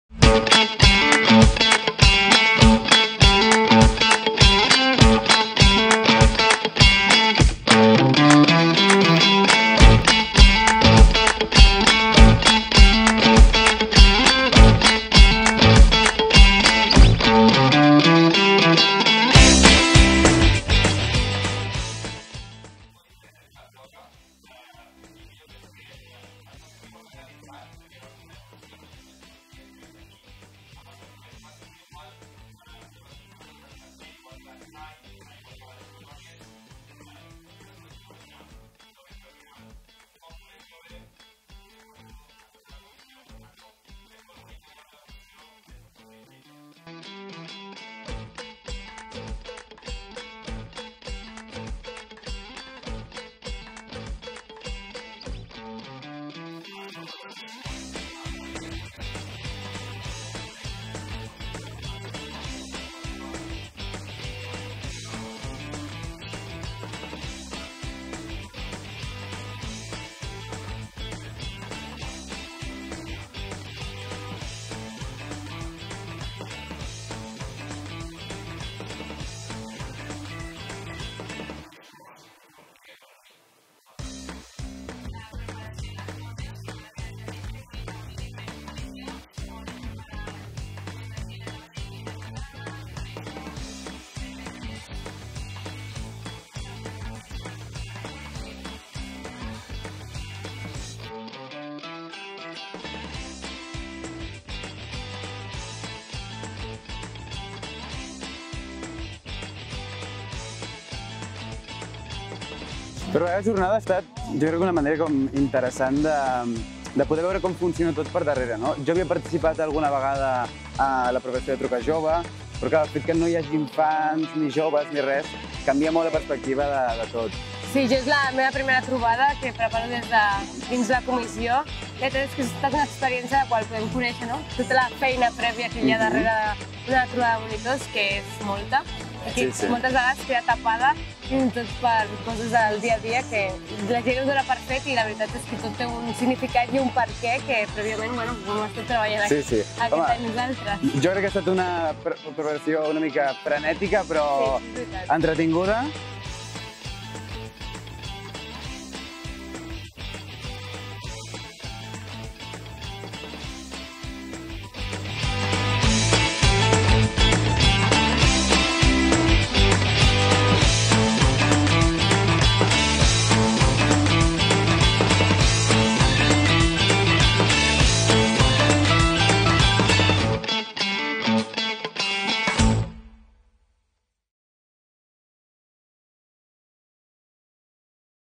Making of Trobades 2022 - Vine a preparar les següents!
Així hem preparat les trobades d'aquest any. L'any que ve ens hem de superar. Vine a preparar les trobades 2023!!!